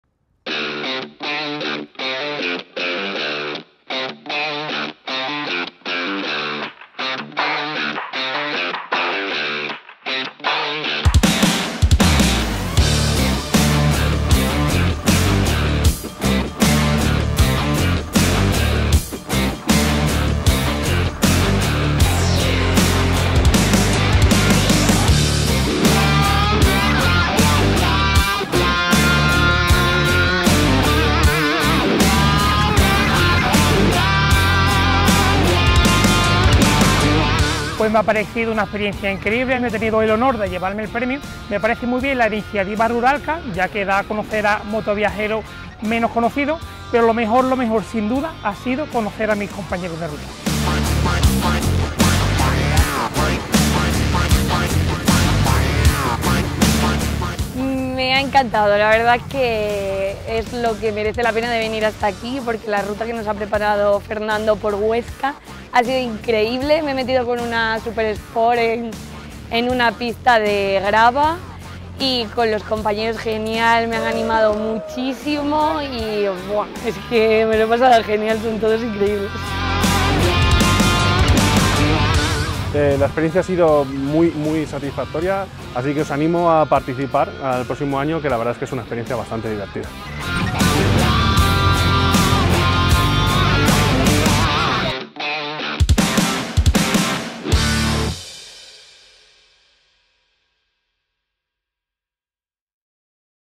IV Edición Cuna de Viajeros
Video realizado para Ruralka en colaboración con BMW Motorrad España, en el que se muestra un resumen de la 4ª Edición de Cuna de Viajeros.